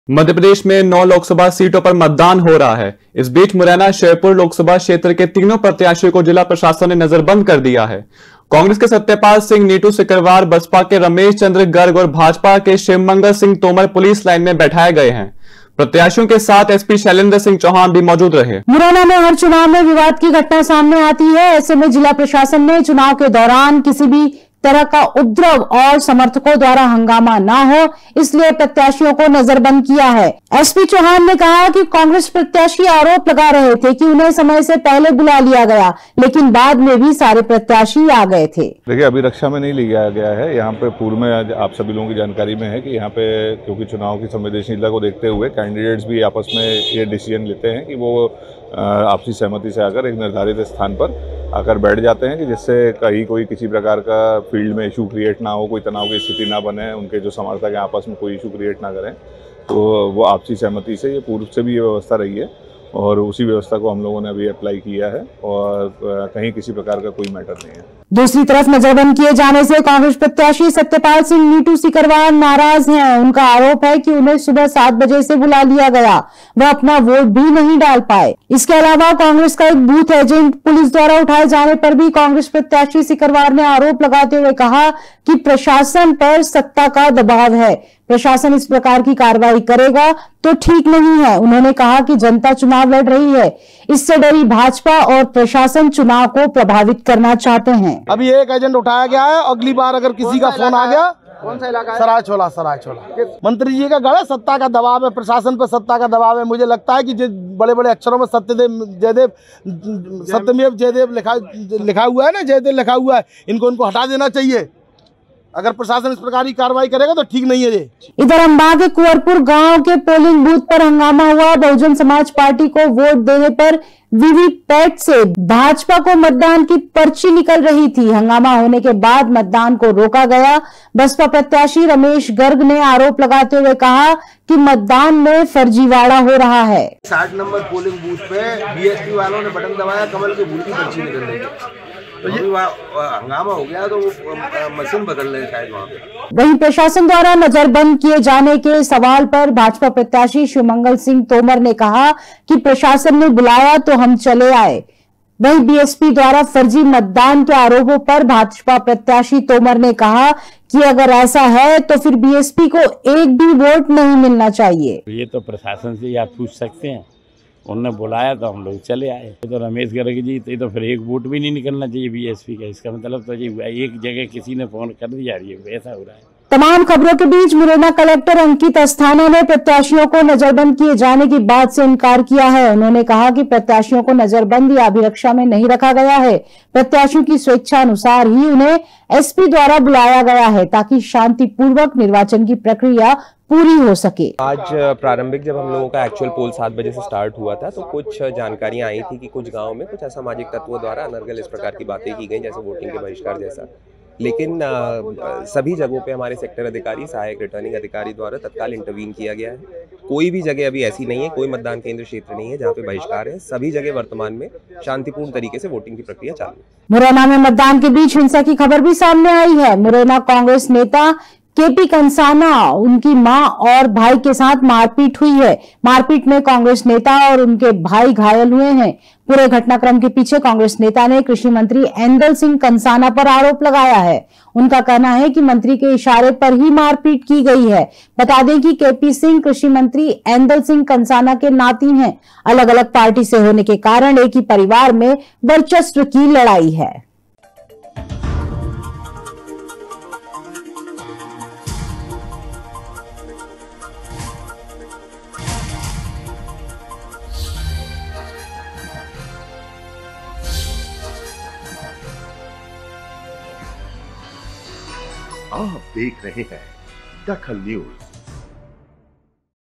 DAKHAL NEWS तीनों नेताओं को पुलिस लाइन में बैठाया गया
तीनों नेताओं को पुलिस लाइन में बैठाया गया
चुनाव में उपद्रव और हंगामा रोकने के लिए प्रशासन का फैसला